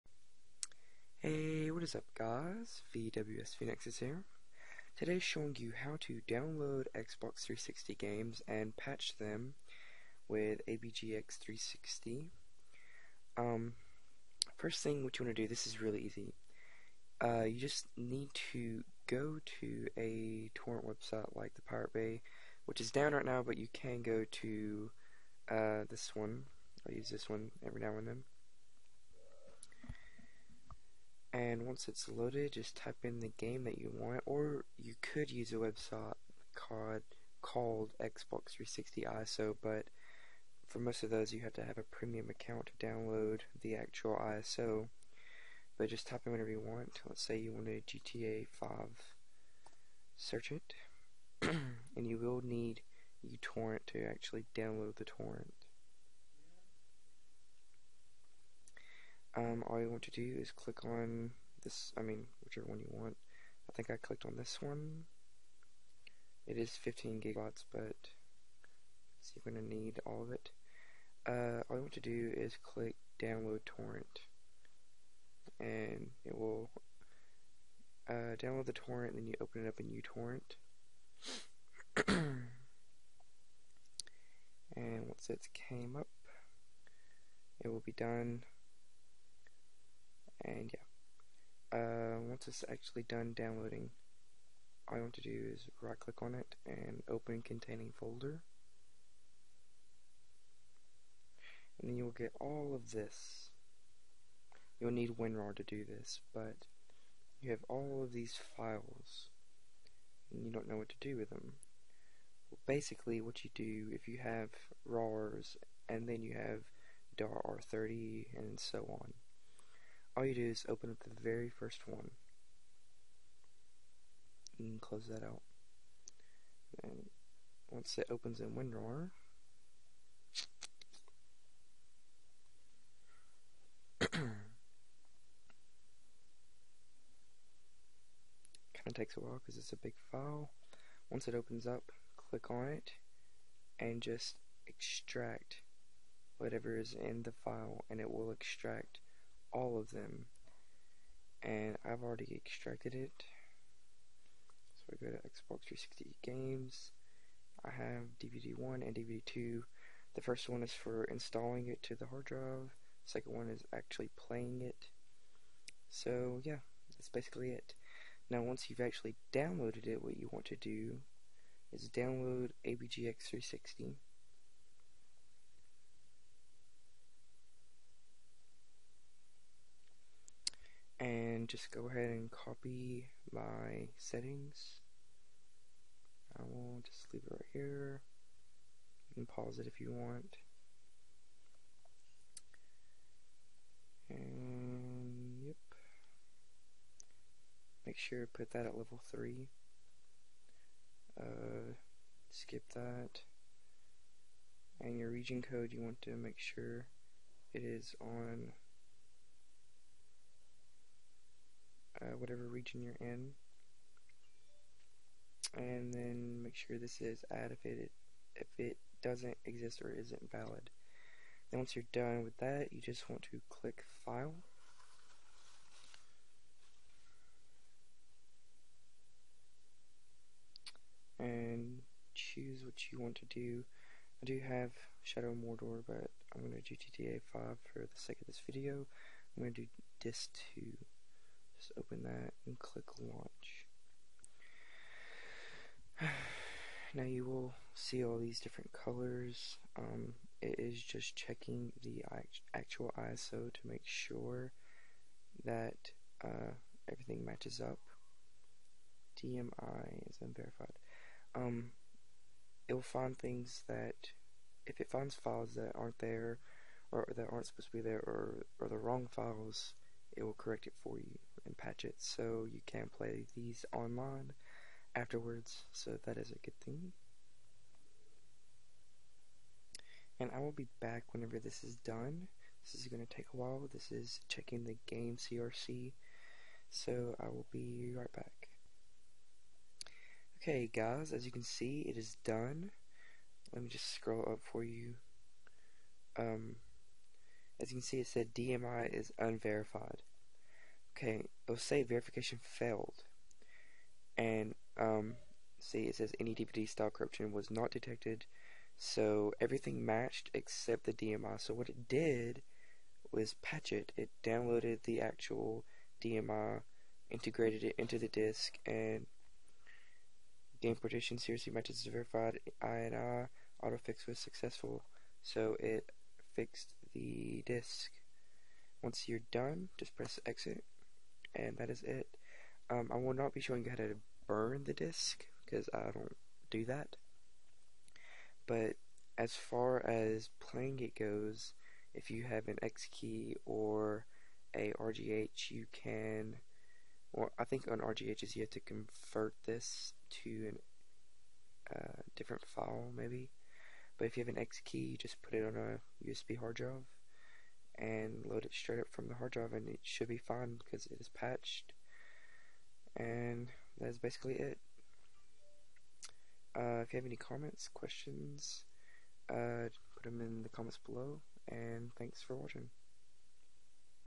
How to download and patch Xbox 360 ISO's with abgx360
Hey guys!

Today I am going to show you how to download and patch 360 ISO's to play on your Xk3y or RGH. Yes this is Xbox Live Safe.

Here are the programs that you will need.

(For abgX360 make sure you download the file from the homepage of their website and put it in the folder they tell you to, other wise it will not patch correctly)